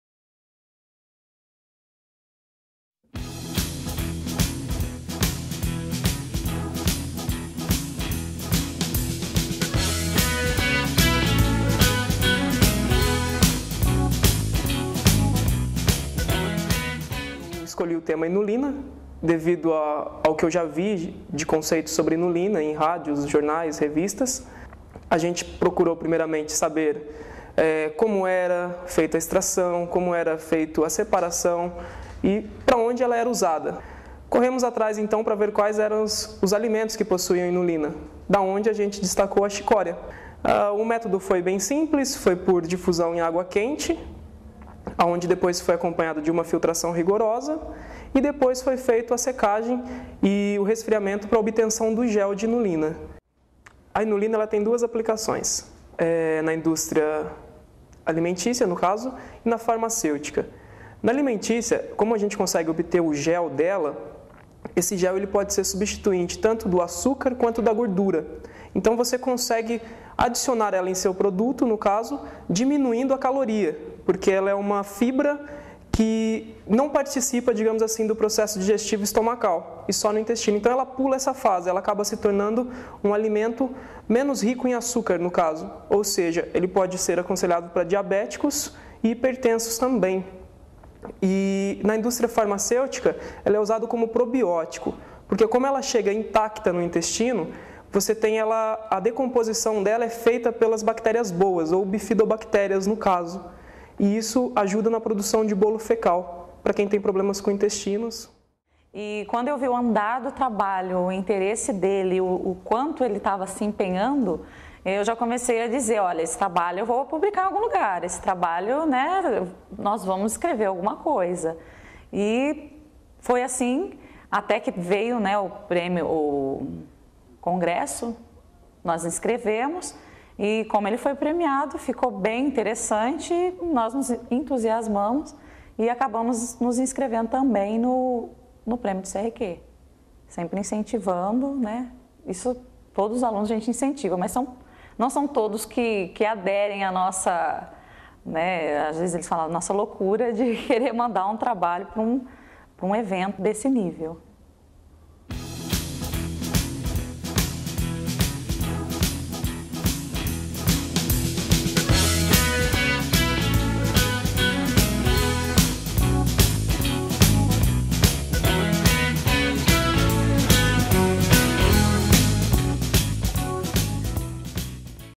Prêmio CRQ-IV - Química de Nível Superior com Tecnologia
Ganhador na modalidade Química de Nível Superior com Tecnologia da edição 2012 do Prêmio CRQ-IV, aluno da Fundação Educacional do Município de Assis e sua orientadora dão detalhes do trabalho sobre a criação de um gel para uso na indústria alimentícia e farmacêutica extraído da raiz da chicória.